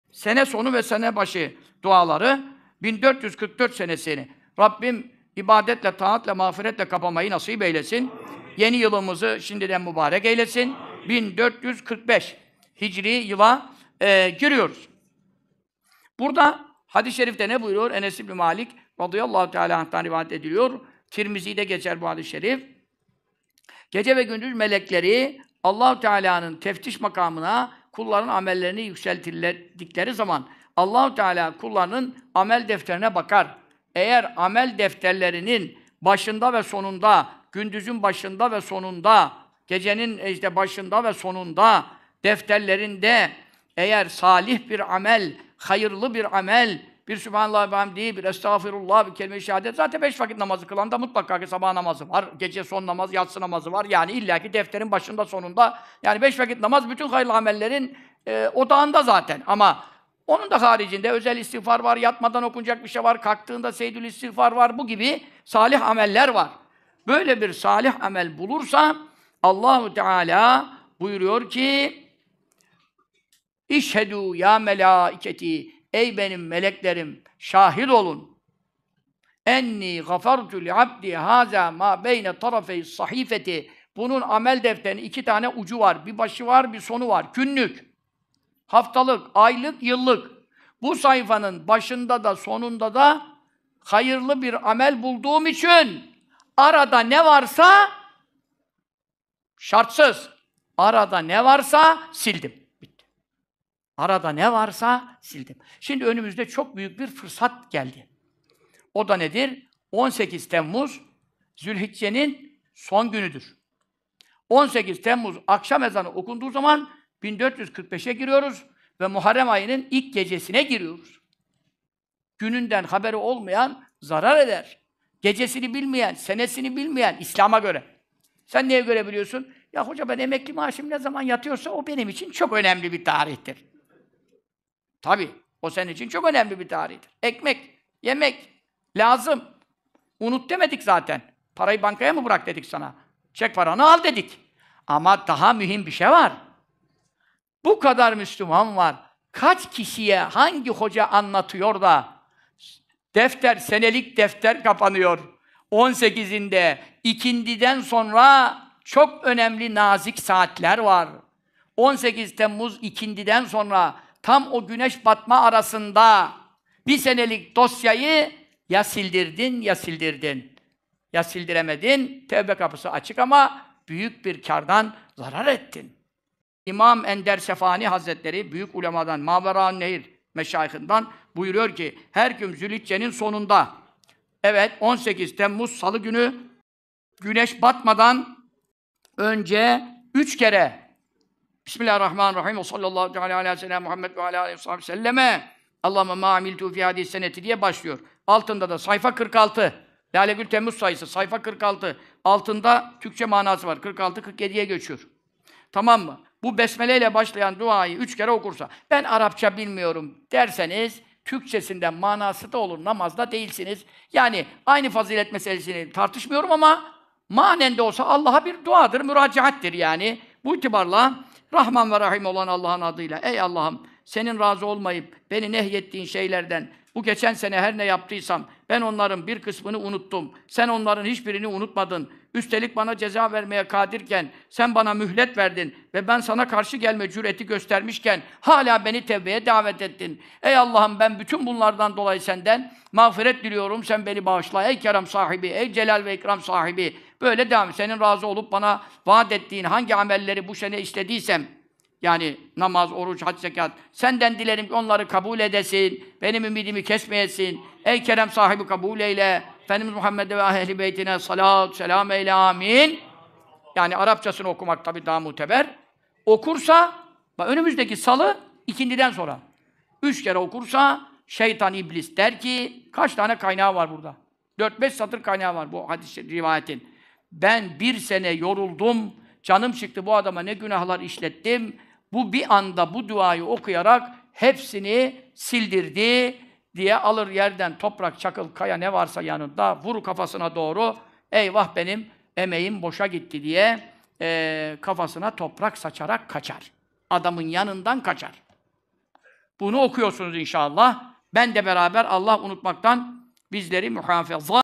Hicri yılbaşında okunacak sene sonu ve sene başı duası.. duanın okunuşu açıklama bölümünün linkinde👇
Hocamızın bahsettiği sene sonu ve sene başı duasını Türkçe meali ile birlikte Aşağıdaki linkten bulabilirsiniz👇